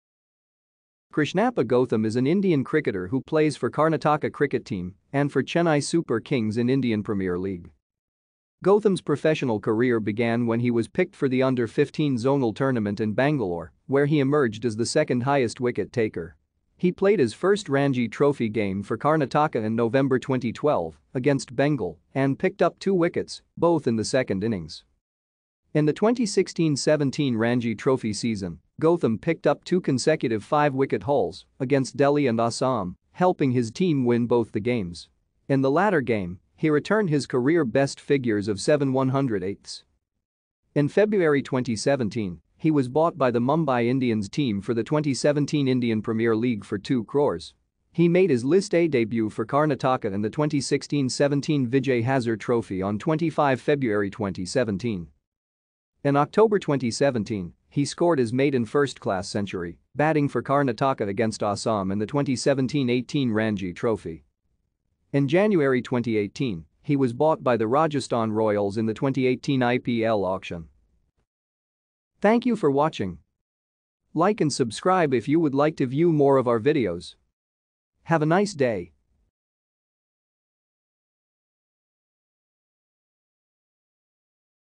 Krishnappa Gowtham
Krishnappa Gowtham  is an Indian cricketer who plays for Karnataka cricket team & for Chennai Super Kings in Indian Premier League.